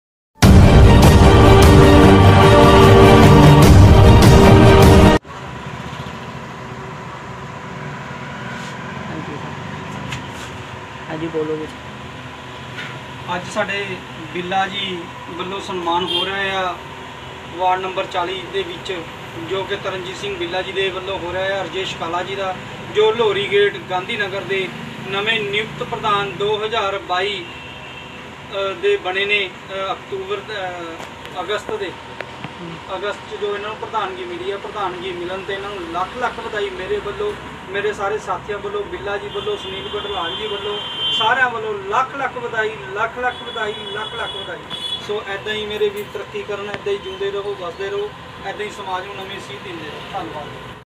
ਅੱਜ ਮਿਤੀ12 .8.2022 ਨੂੰ ਪ੍ਰਧਾਨ ਸੁਨੀਲ ਲਹੌਰੀ ਗੇਟ ਕਈ ਨਗਰ ਦਾ ਵਿਸ਼ੇਸ਼ ਤੌਰ ਤੇ ਸਨਮਾਨ ਕੀਤਾ ਗਿਆ ।
@ਪੰਜਾਬ ਫੋਕਸ